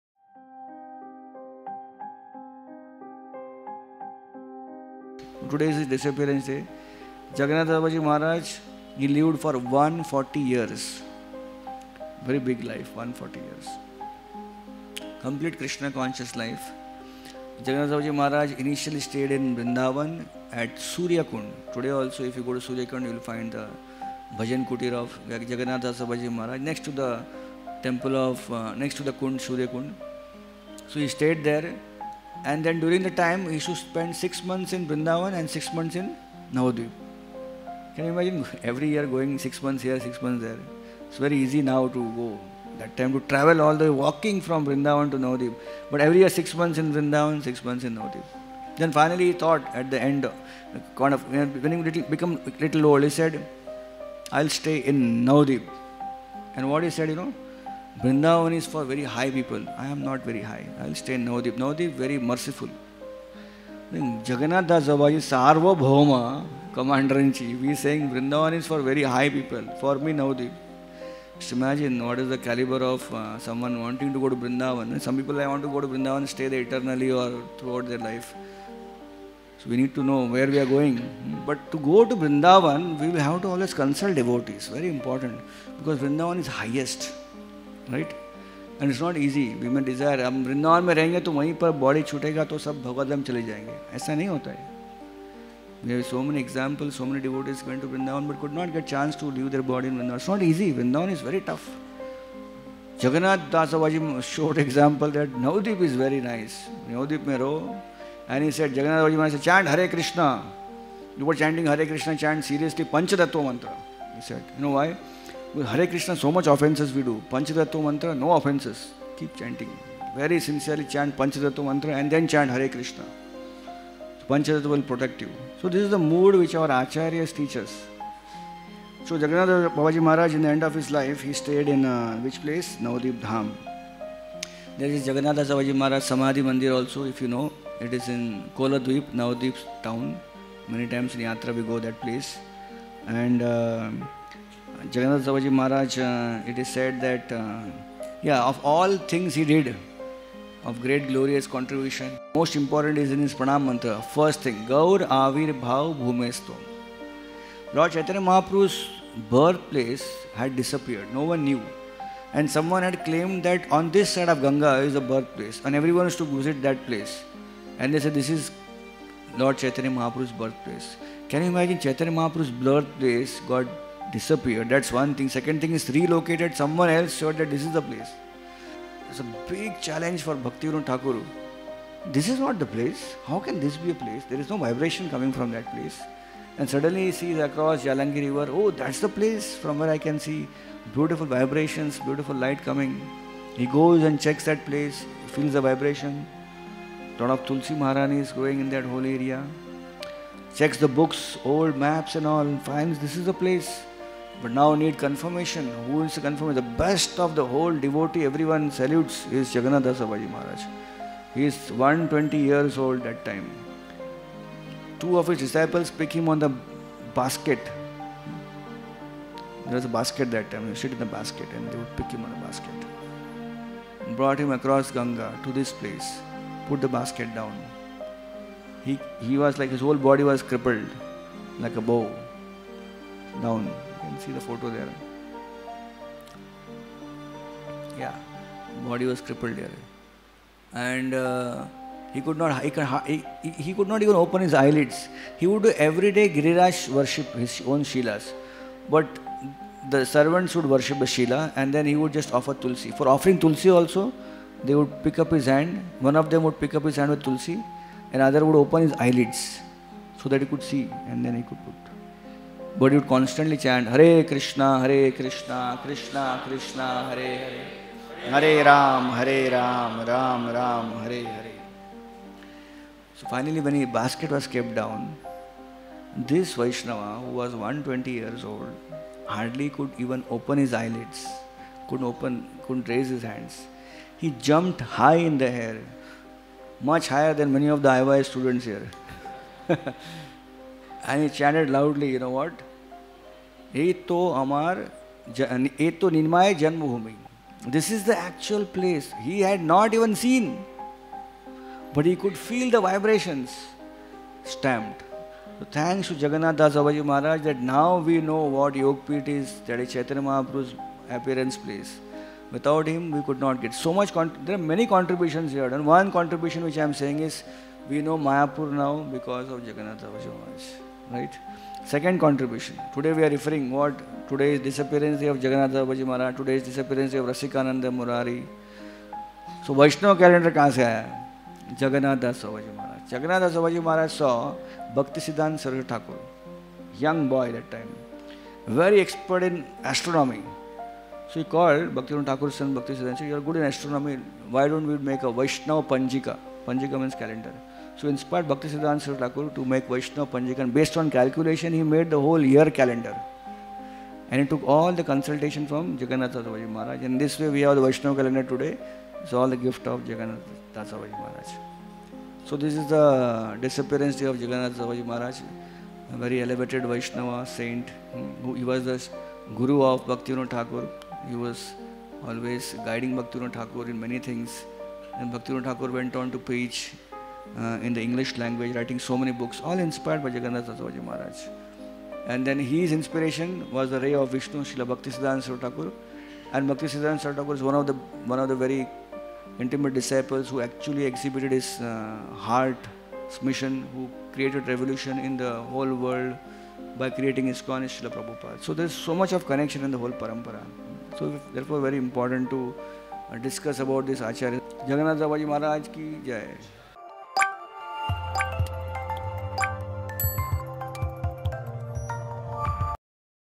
Glories of Jagannath Das Babaji | Baladev Das | ISKCON Chowpatty
Remembering Srila Jagannath Das Babaji Maharaj on his disappearance day at ISKCON Chowpatty on 14 Mar 2021